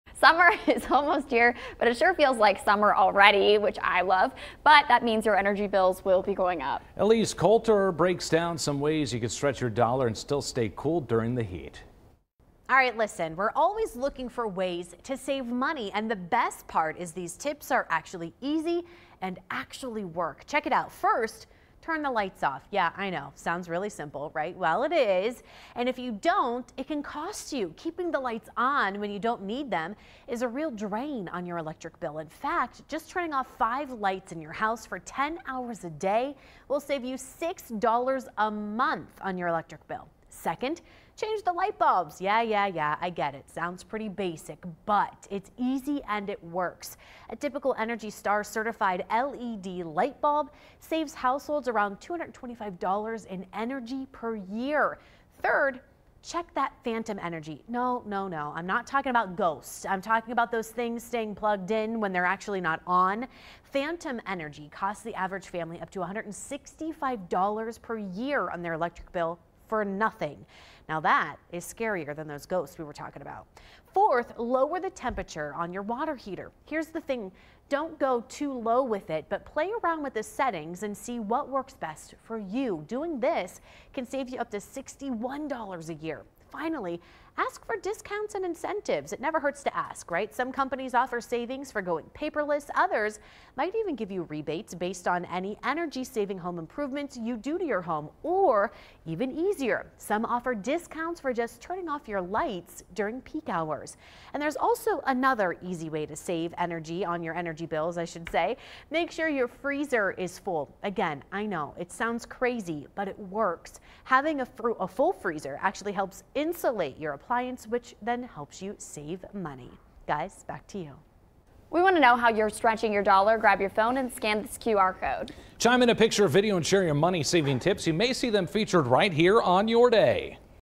Stretching Your Dollar: Saving energy this summer
Summer is almost here, but with higher temperatures comes higher energy bills.
Elyse Coulter shares tips to save on energy this summer.